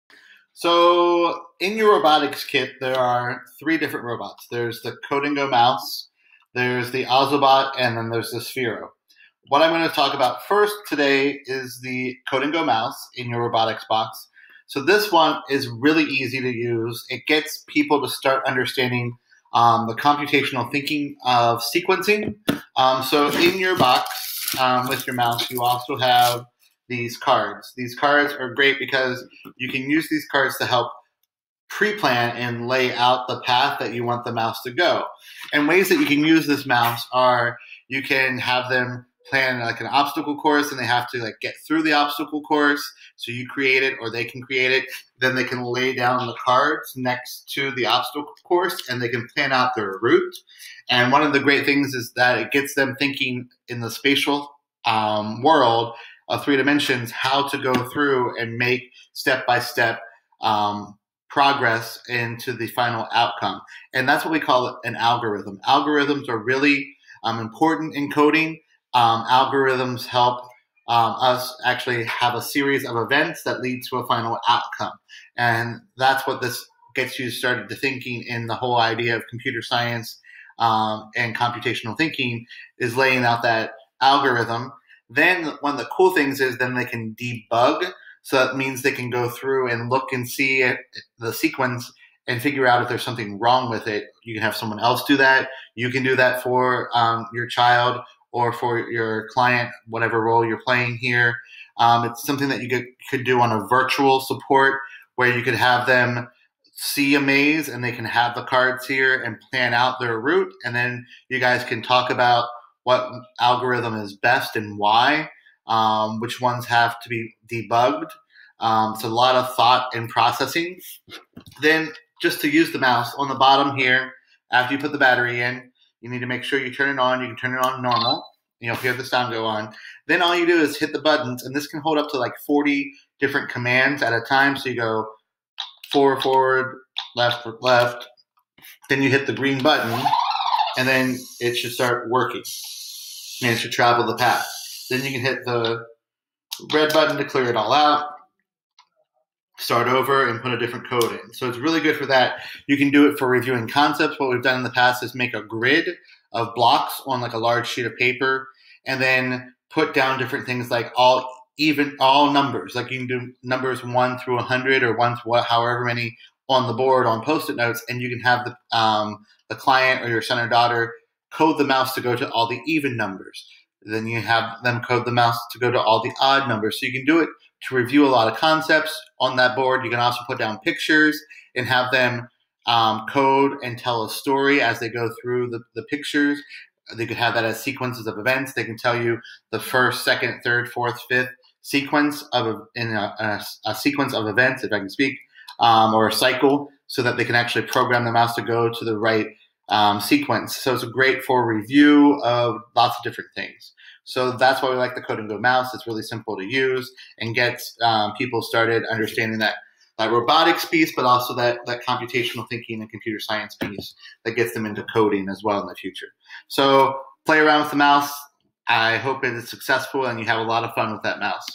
Getting Started with Robotics Code & Go Mouse
This video shows you how to use the Code & Go mouse and begins to show you ways to use the mouse in activities for your son/daughter or client.